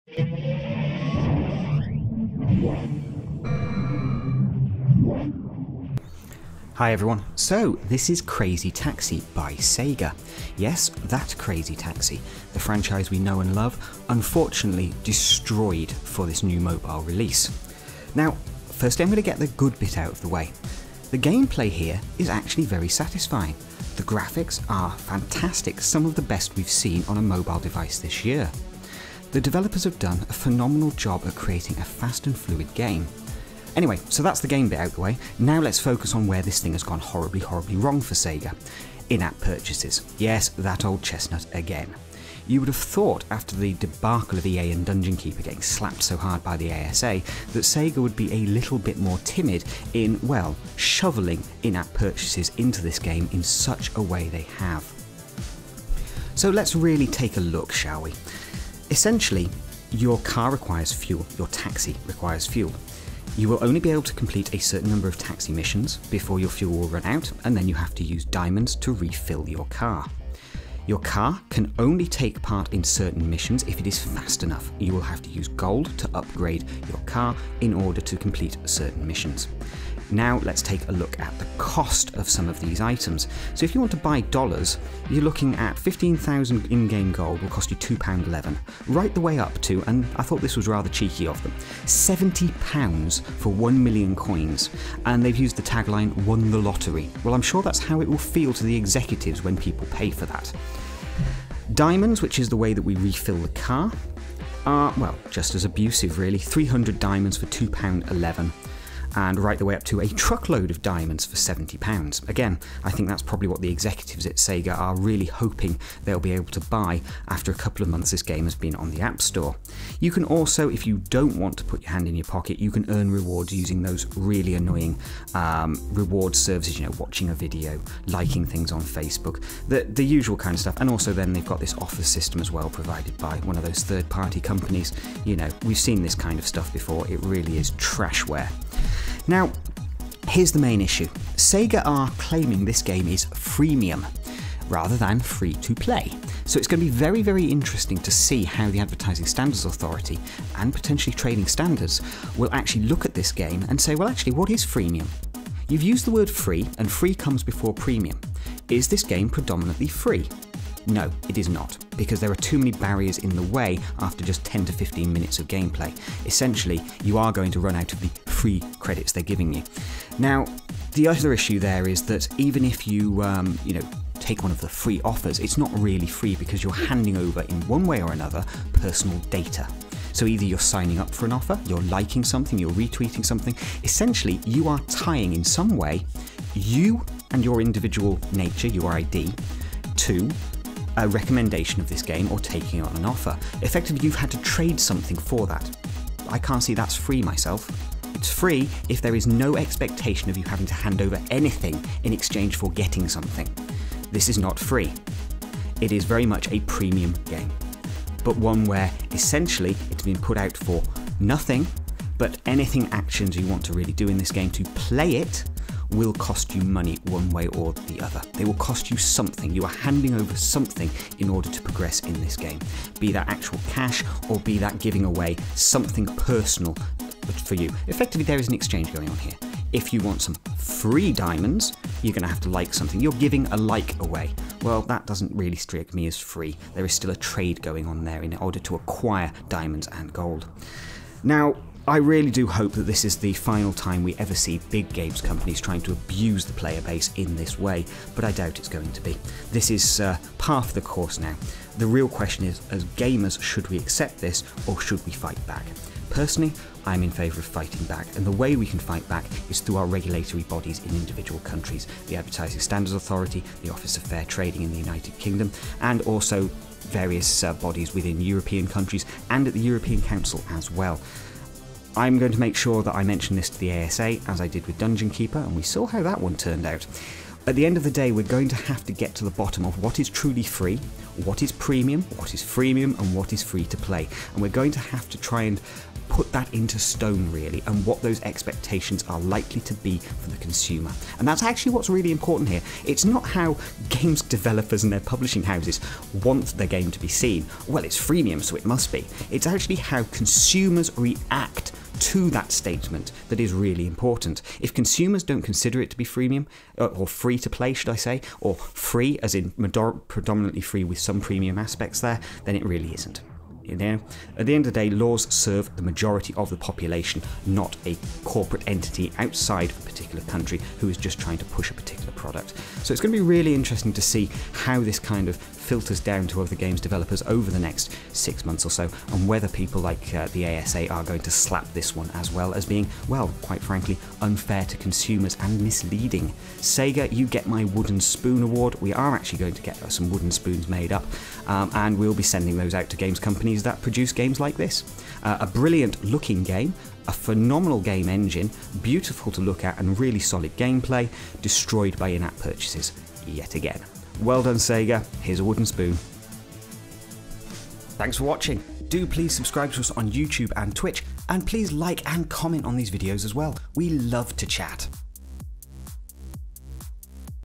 Crazy Taxi City Rush and Sega's abusive IAP's - Androidizen
Crazy Taxi City Rush and Sega's abusive IAP's. Today we take a look at Crazy Taxi City Rush and discuss the abusive IAP's that Sega has shoved into this mobile game. We consider if Freemium is enough to keep this game from being slapped by the ASA and Trading Standards for the way the "game" is structured in relation to expectations of players.

It's time the big publishing houses stopped this abuse of gamers and the general public. If this keeps up the mobile industry will collapse as players lose trust.

The Androidizen